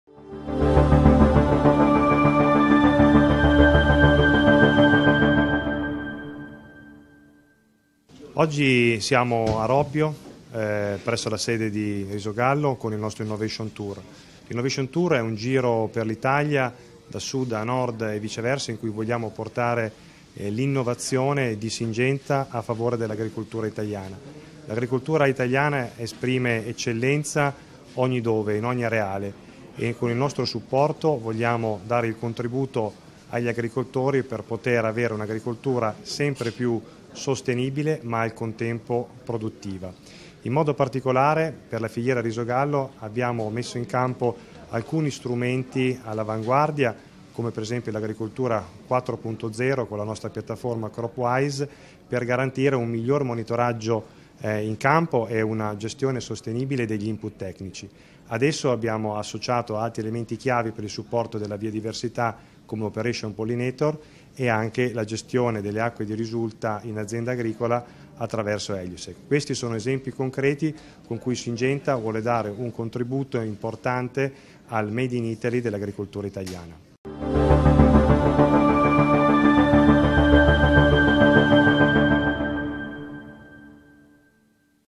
Vanelli (Syngenta): “Portiamo l’innovazione a servizio dell’agricoltura italiana”
Continua il giro d’Italia di Syngenta con il suo Innovation Tour. L’amministratore delegato, Riccardo Vanelli, spiega come sia fondamentale puntare su un’agricoltura innovativa, sostenibile e produttiva.